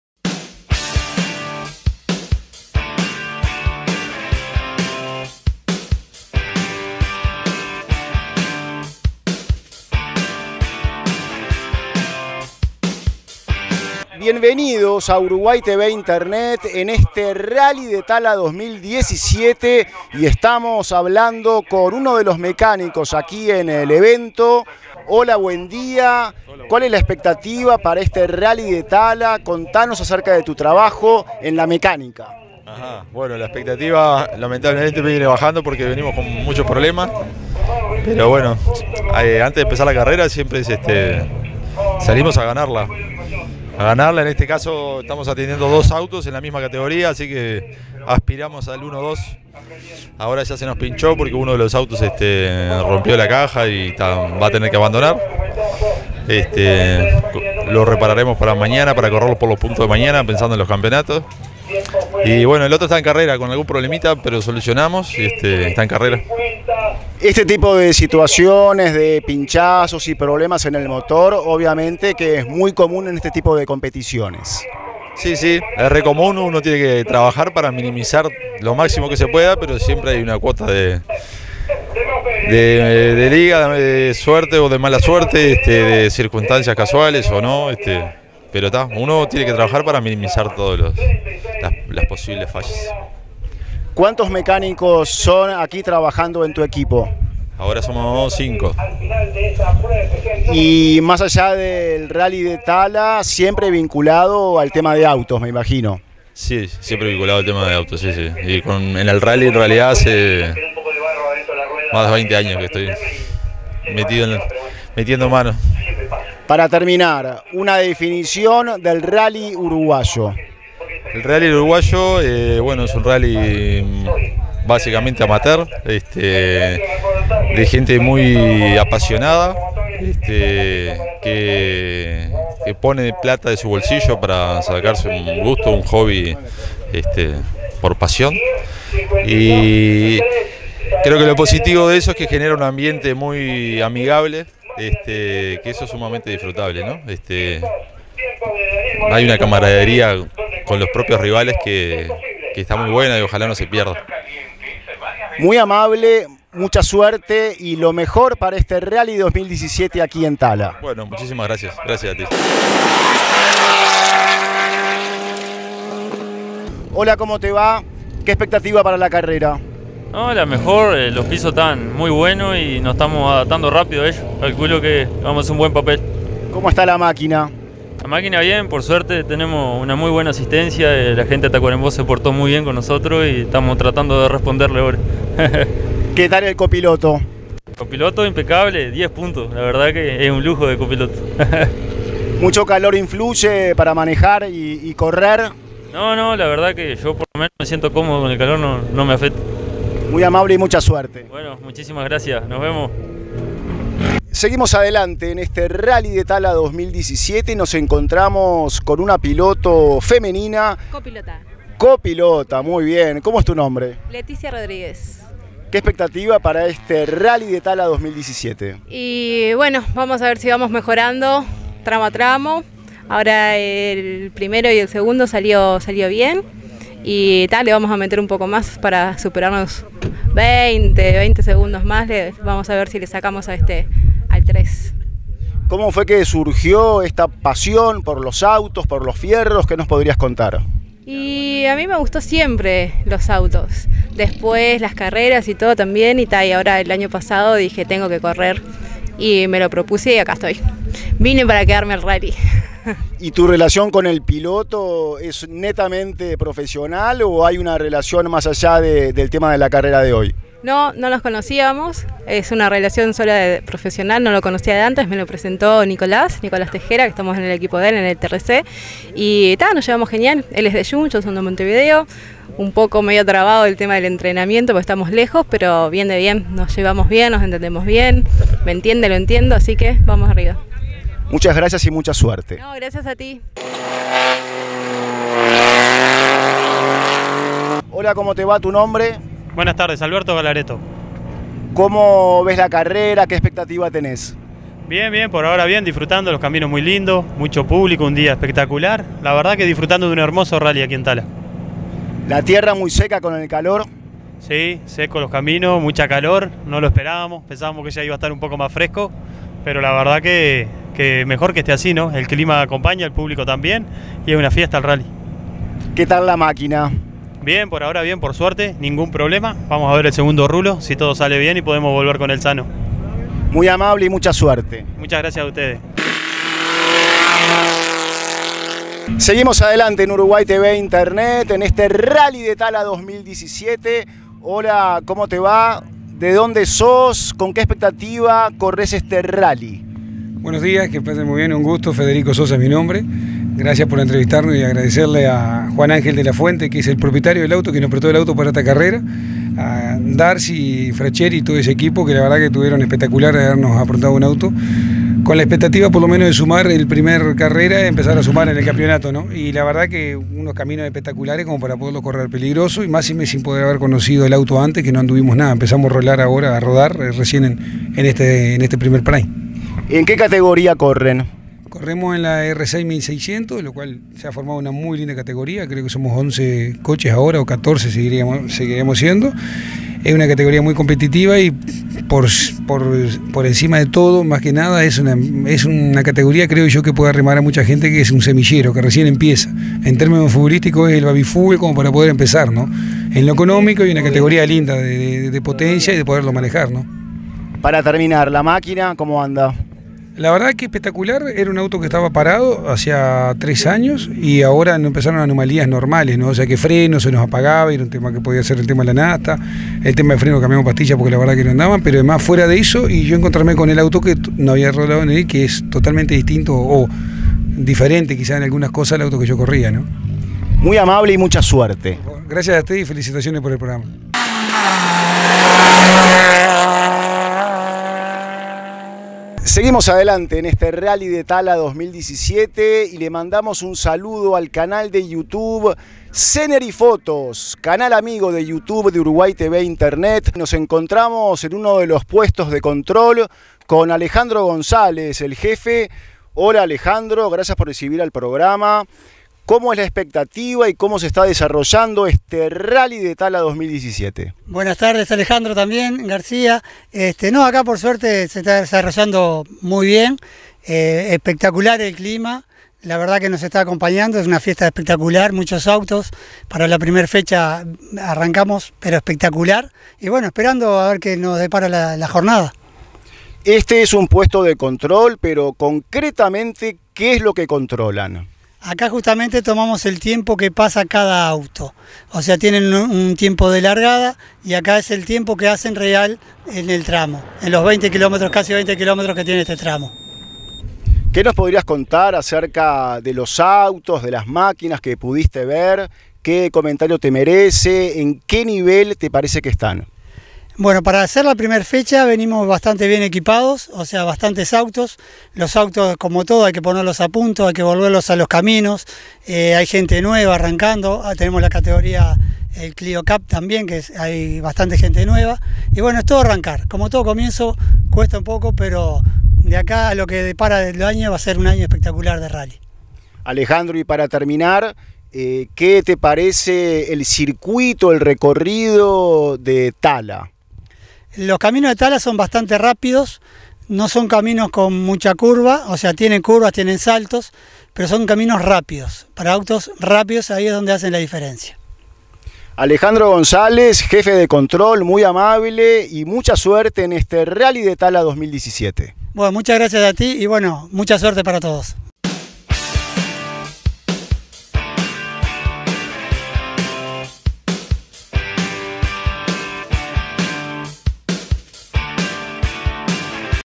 RALLY de Uruguay 2017 ( TALA ). Imágenes y entrevistas.
Imágenes y entrevistas en el Campeonato Uruguayo de Rally realizado en la Localidad de Tala en el Departamento de Canelones, Uruguay -  2017.